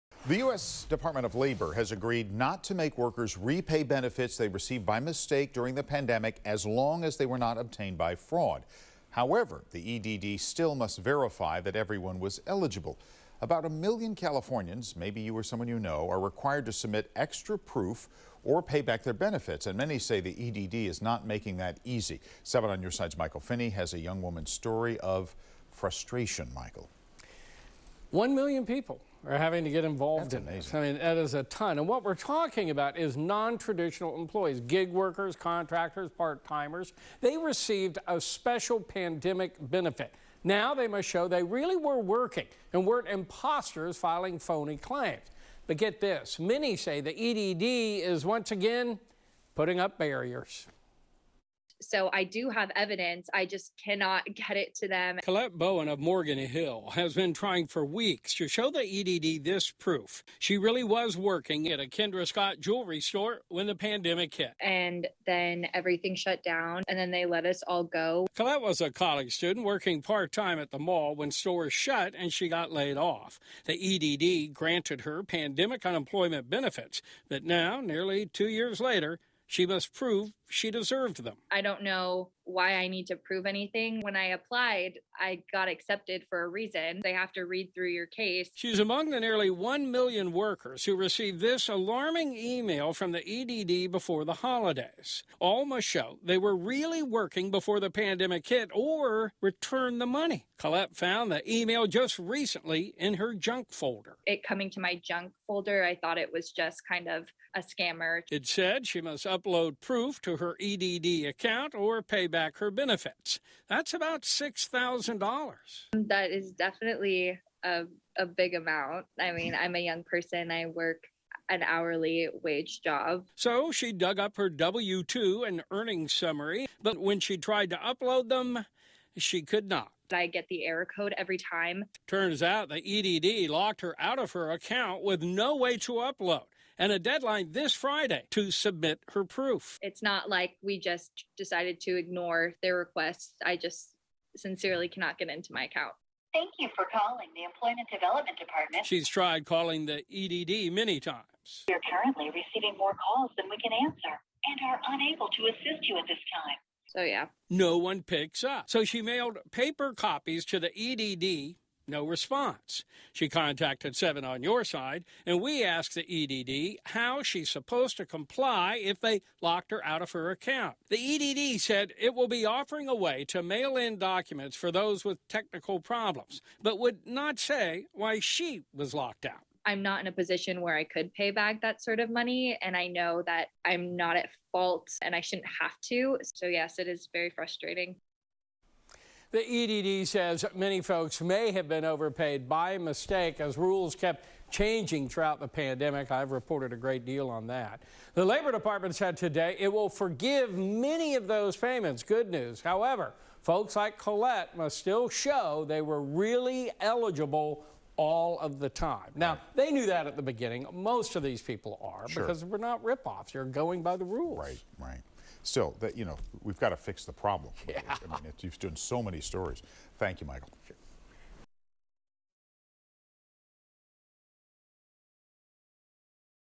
EDD tells young woman to prove she's a real worker, then locks her out
The EDD is making about 1 million Californians prove they qualified for the unemployment benefits they received or pay all the money back. For one young woman, providing proof isn't the issue, it's that EDD locked her out of her account, and the deadline is approaching fast. "I'm not in a position where I could pay back that sort of money, and I know that I'm not at fault, and I shouldn't have to be. It's definitely been stressful," she said.